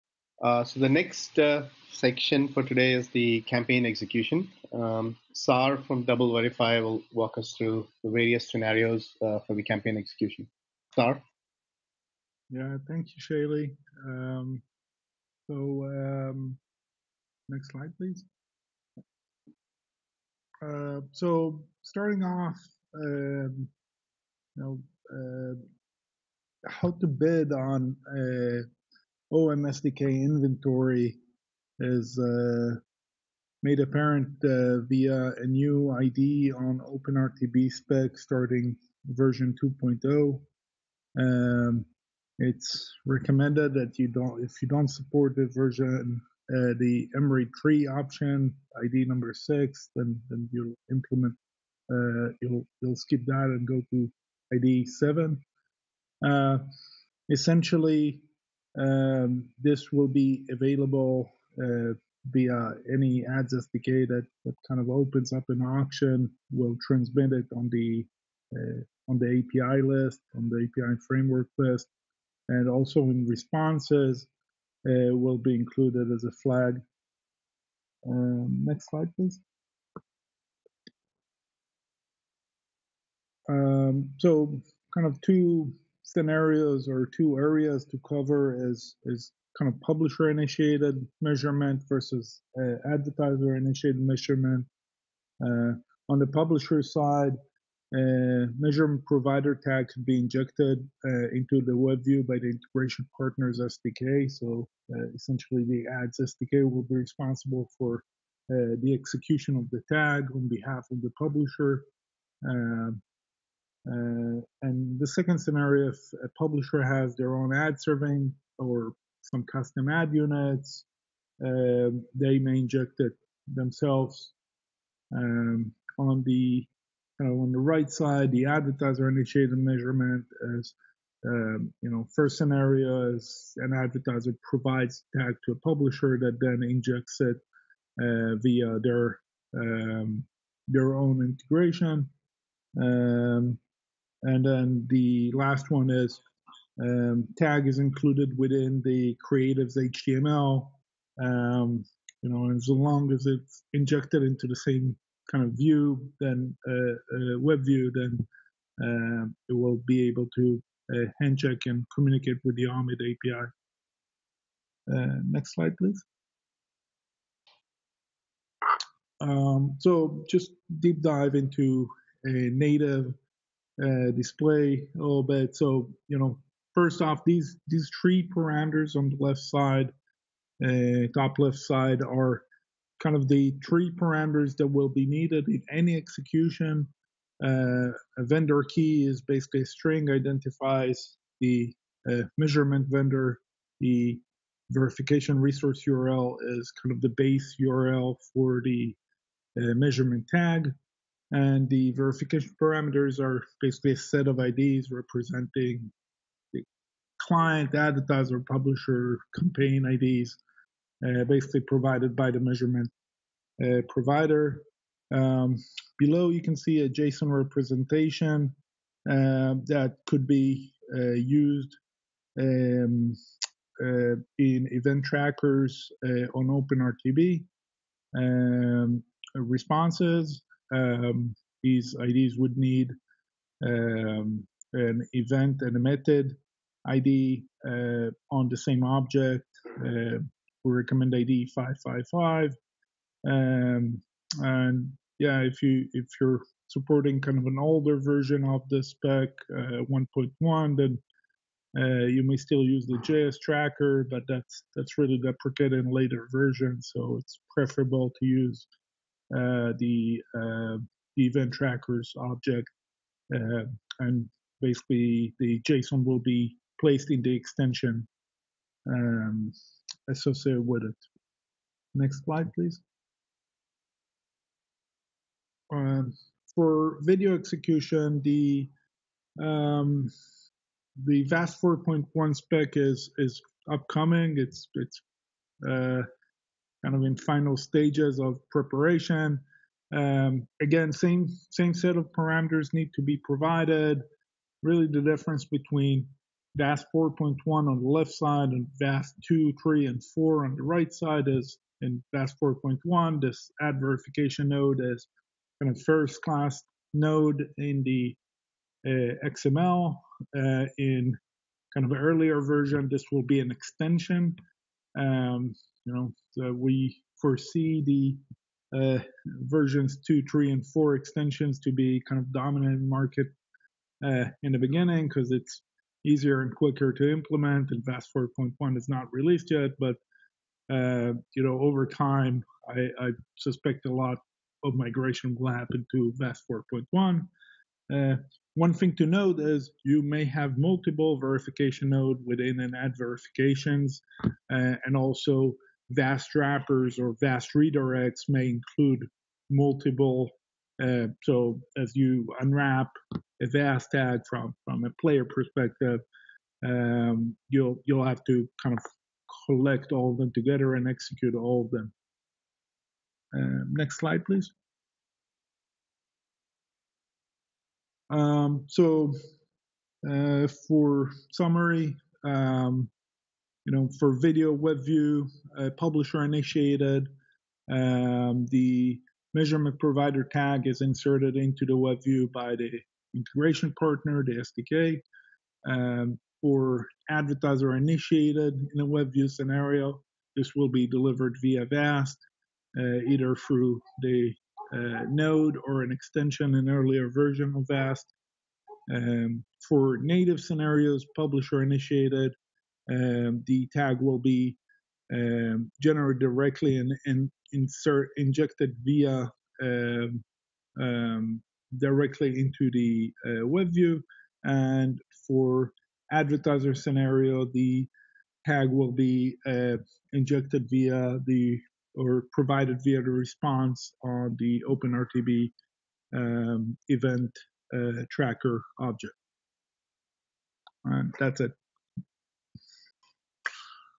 Open Measurement SDK: Show and Tell Webinars: Campaign Execution
IAB Tech Lab recently released the Open Measurement (OM) Software Development Kit (SDK) for general availability. In these webinars, we will provide in depth information about the Open Measurement SDK- how it works, how to integrate in your app or Ad SDK and how to enable different types of ads.